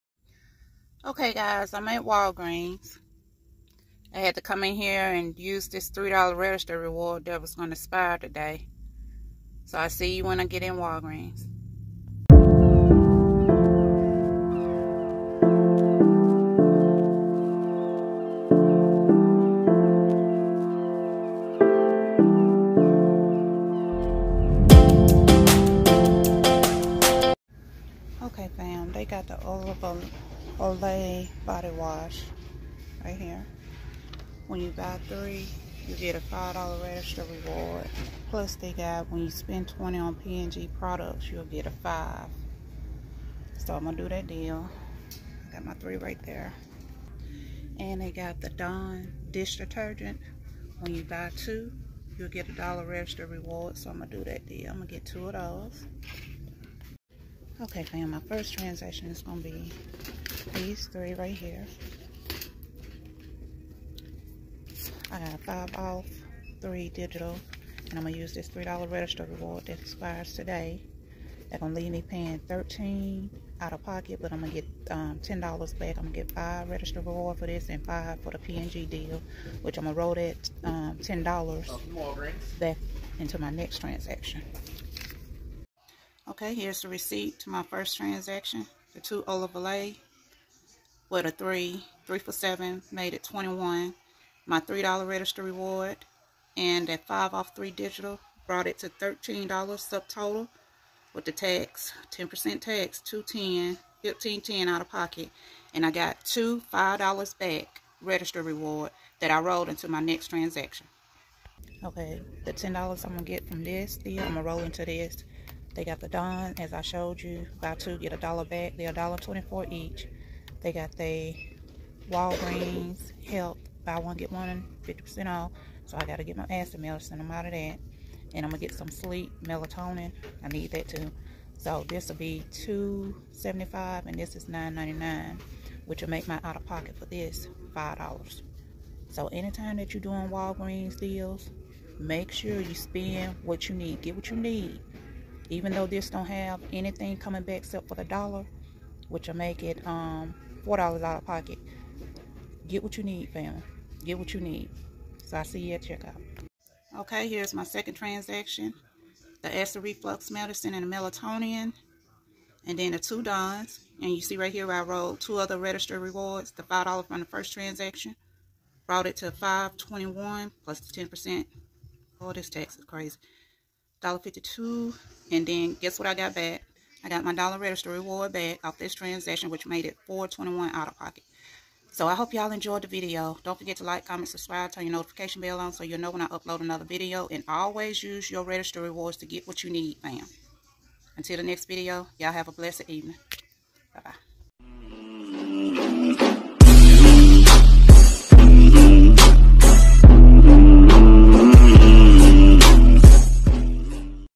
Couponing At Walgreens: Easy Deals For Beginners
Couponing at Walgreens: Easy Deals For Beginners.🎉✨ Dive into the world of couponing with us as we explore the aisles of Walgreens, revealing the easiest deals for beginners.

Timecodes
0:00 Intro
0:28 Olay Deal
0:47 Dawn Deal
1:00 1st transactions
1:53 2nd transaction